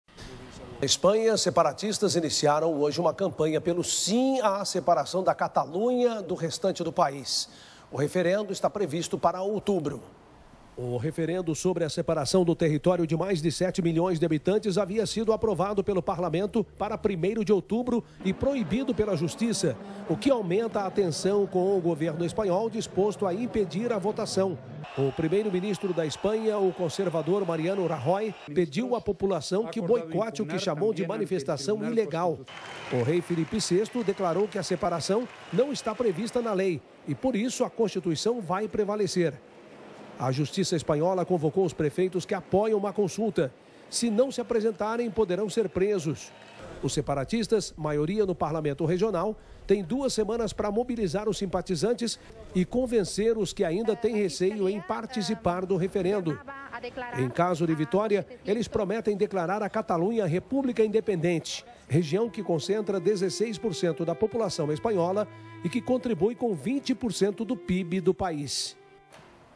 Na Espanha, separatistas iniciam campanha pela separação da Catalunha do restante do país
Na Espanha, separatistas iniciaram, nesta quinta-feira, 14, uma campanha pelo "sim", à separação da Catalunha do restante do país.  O referendo está previsto para Outubro.